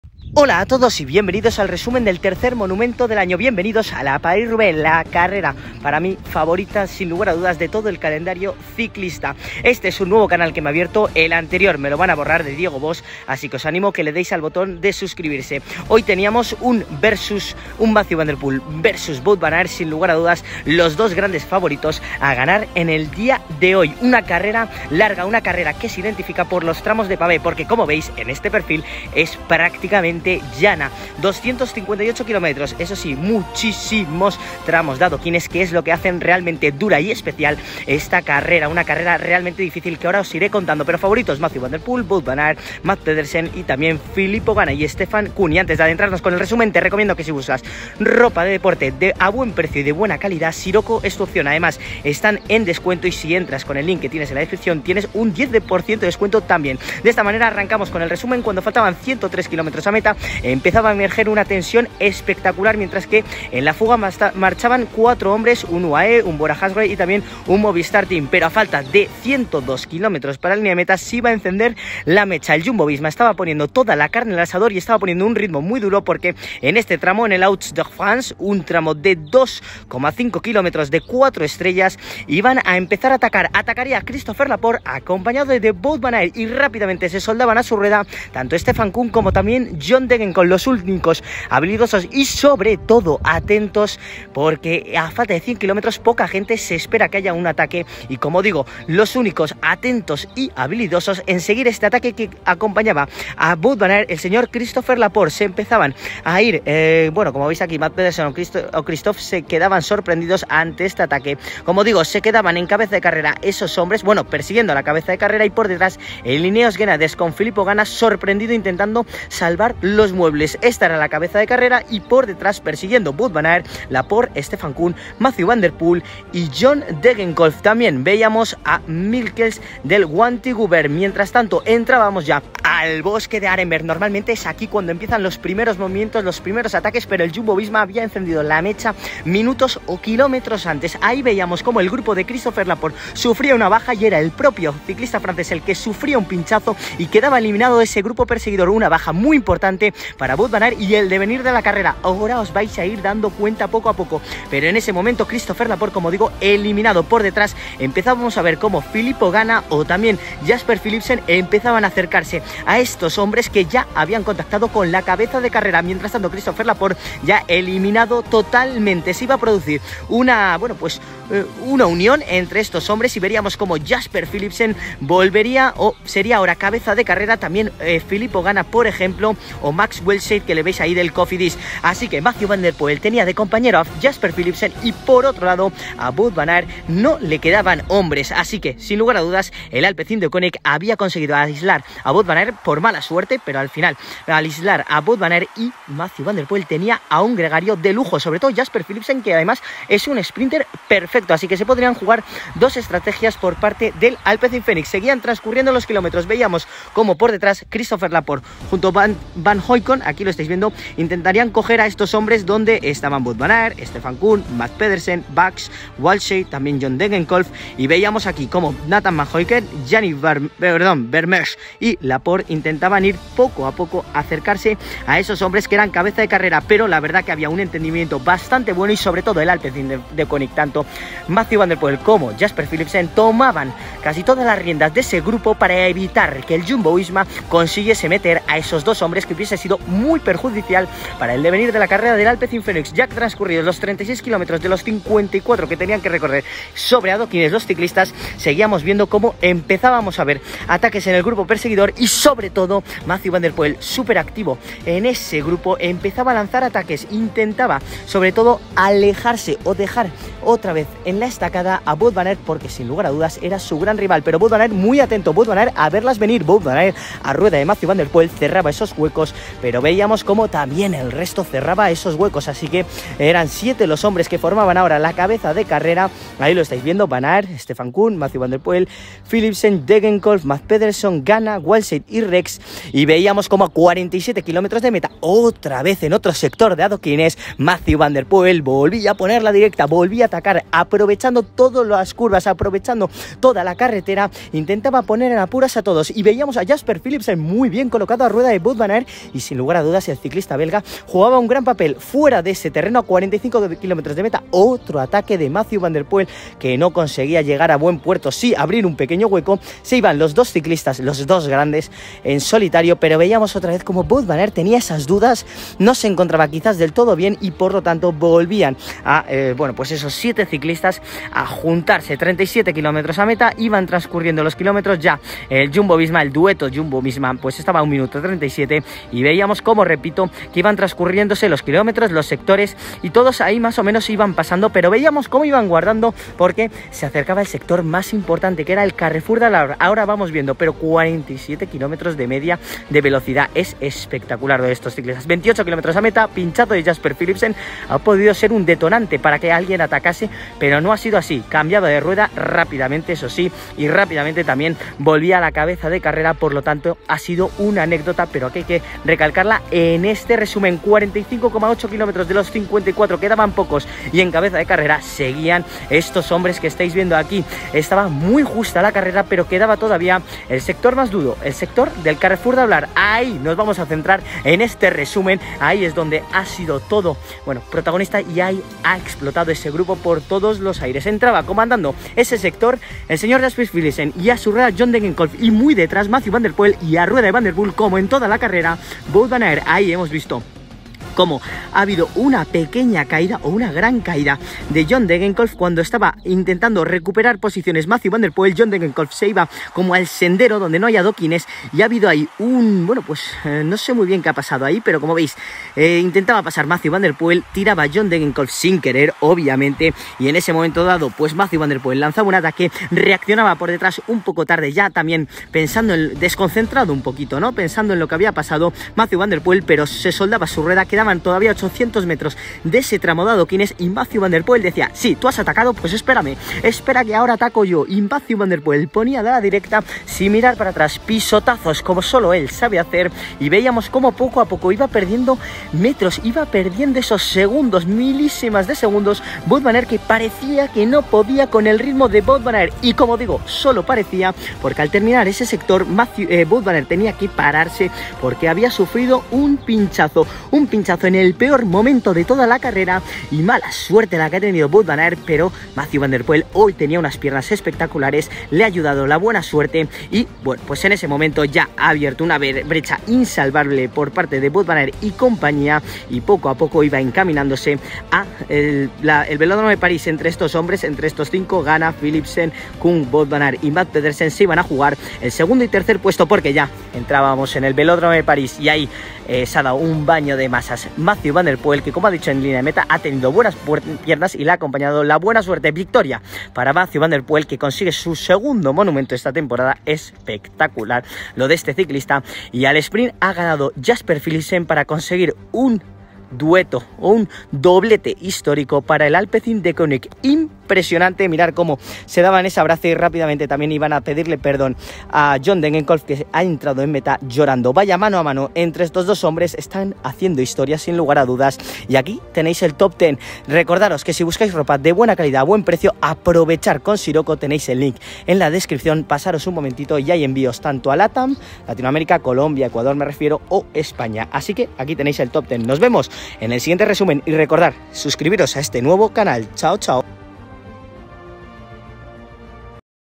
RESUMEN PARIS ROUBAIX 2023 - BIENVENIDOS AL INFIERNO DEL NORTE
Mathieu van der Poel entró en solitario en el Vélodrome André-Pétrieux para continuar su asalto a los libros de historia, ganando la París-Roubaix por primera vez en su tercer intento.

Fue una actuación típica del holandés, que se hizo con el cuarto título del Monumento de su carrera, pero la sensación de "qué hubiera pasado si" siempre estará presente en esta carrera, al menos en la mente de Wout van Aert (Jumbo-Visma).

Van Aert había abierto la carrera con más de 100 km de ventaja, y estaba haciendo su apuesta por la gloria en el último sector de cinco estrellas de Carrefour de l'Arbre, cuando el aire empezó a agotarse de sus neumáticos.

Van der Poel abrió hueco y se alejó de su antiguo rival, que tuvo que parar para cambiar de bicicleta antes de volver al grupo perseguidor, al que atacó sin descanso en la recta final hacia Roubaix.